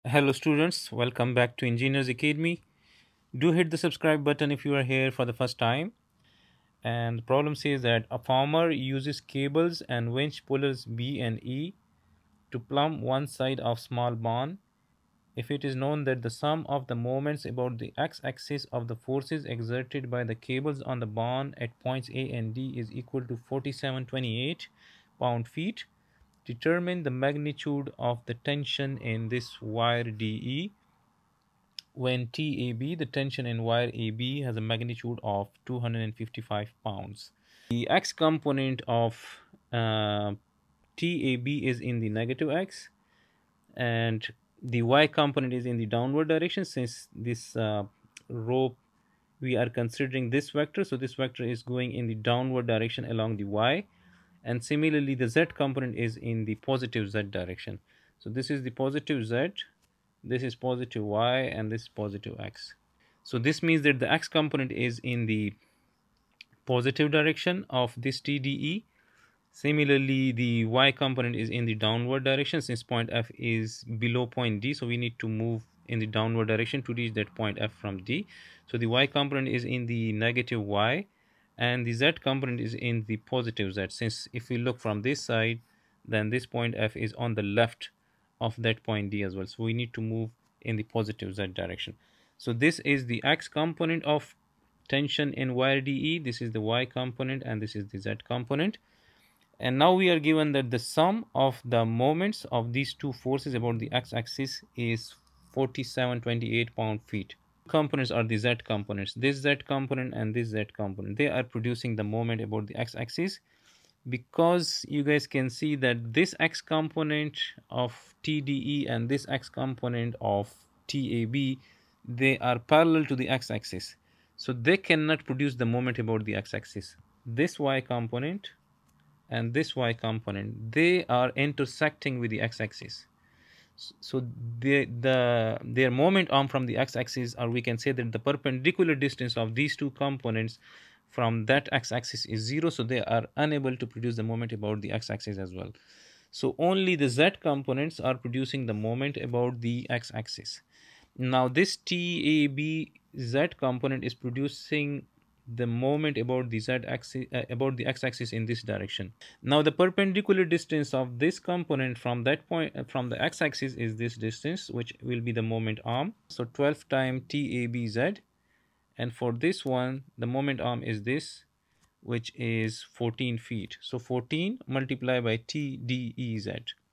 Determine the magnitude of tension in DE | Vector Mechanics Beer & Johnston | Engineers Academy
🎓 Vector Mechanics Problem 3.49 | Maximum Tension in Cable ABAD | Statics Moment About z-Axis

📌 Topics Covered:

Position and force vectors
Cross product to determine moment
Extracting the z-component of a moment
Solving inequality for maximum allowable tension

🧮 Problem Statement:

3.51 A farmer uses cables and winch pullers B and E to plumb one side of a small barn. If it is known that the sum of the moments about the x axis of the forces exerted by the cables on the barn at points A and D is equal to 4728 lb • ft, determine the magnitude of TDE
when TAB = 255 lb.
3.52 Solve Prob. 3.51 when the tension in cable AB is 306 lb.

🔧 Tools Used:

Vector algebra
Cross product
Static equilibrium principles
📘 Textbook Reference:
Vector Mechanics for Engineers: Statics by Beer, Johnston, et al. – Problem 3.50

📢 Got a problem you'd like us to solve? Drop it in the comments!